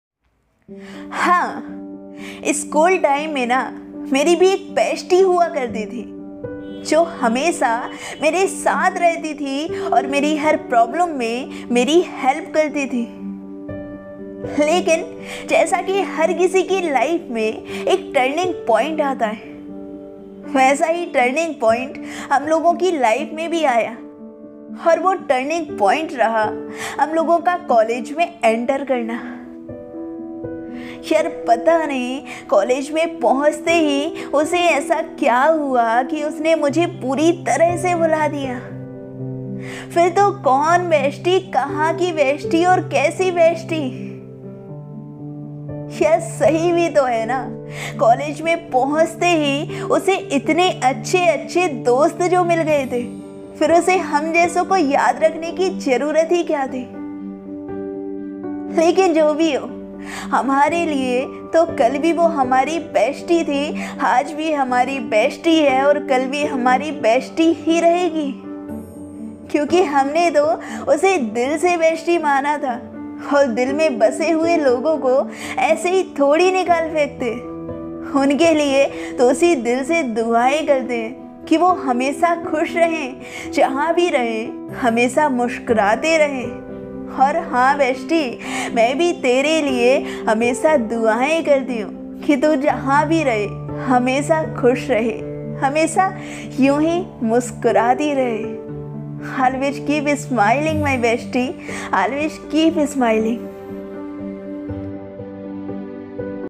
'Best Friend' Change in Collage Time | Female Bestie Poetry | Miss you Bestie Poem | Umang !
Insta I'd - Umang Ek Nayi Pahal

Thank you so much for your love and support also 🙏☺️❤️